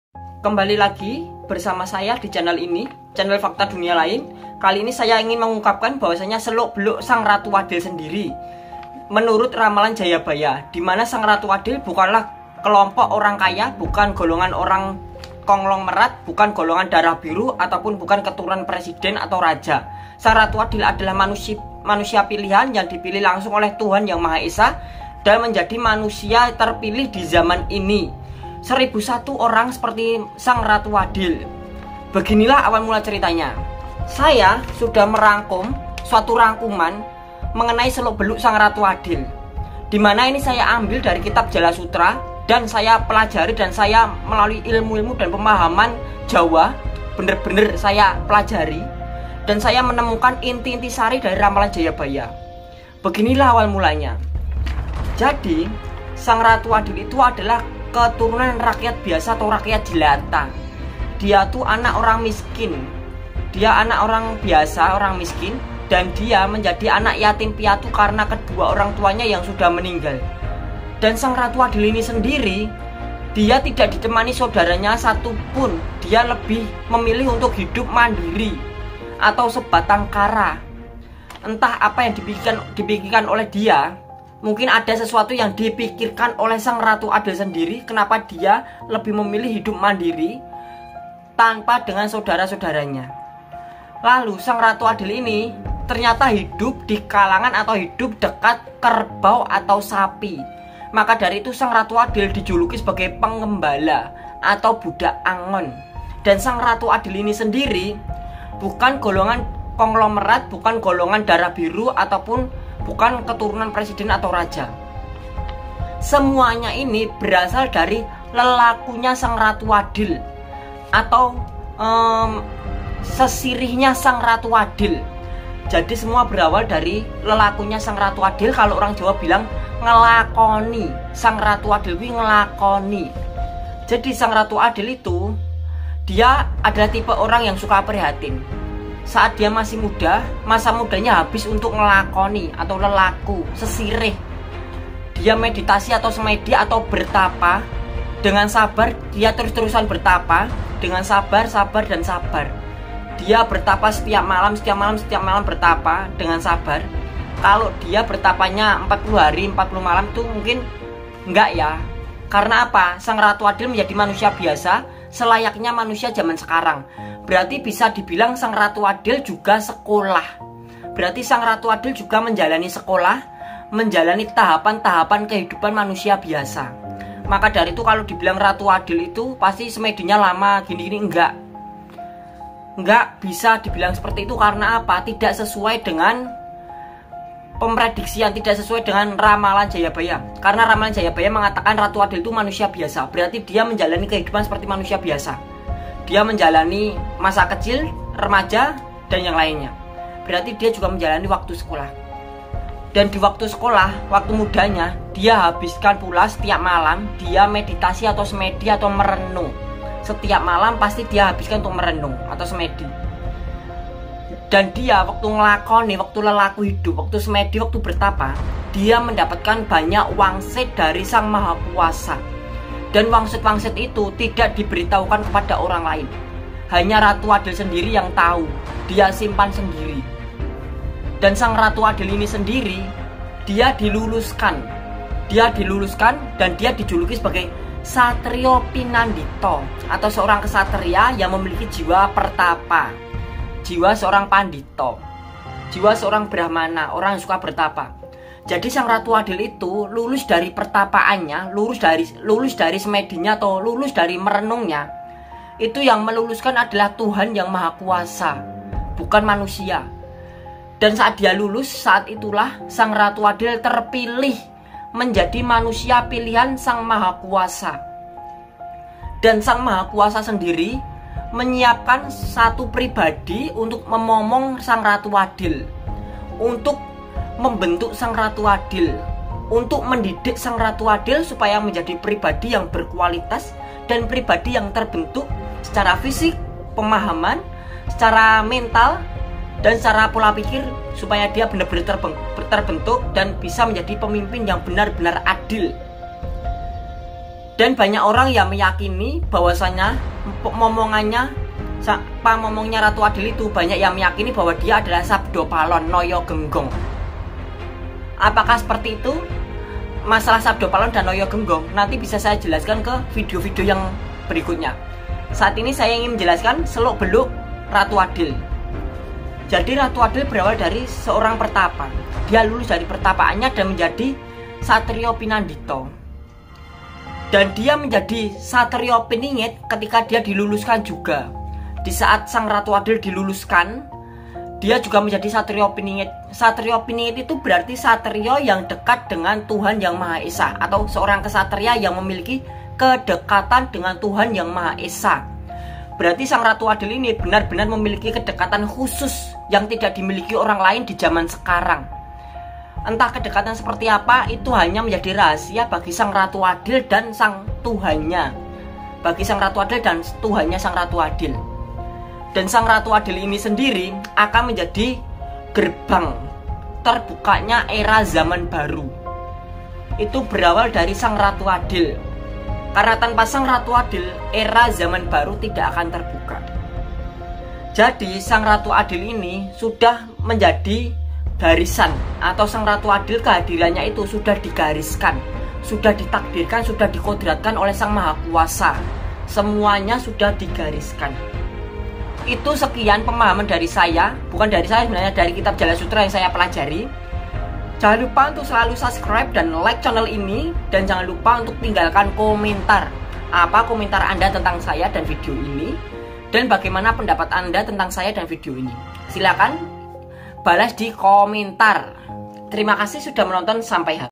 Seluk Beluk Ratu Adil Asli‼️Menurut Ramalan Jayabaya
Seluk Beluk Ratu Adil Asli‼️Menurut Ramalan Jayabaya#ratuadil
Vidio ini menjabarkan mengenai seluk beluk ratu adil yang sebenarnya sesuai ramalan jayabaya.
Semoga bermanfaat dan menghibur kalian semua.
Dan jangan lupa tinggalin komentarmu di kolom komentar.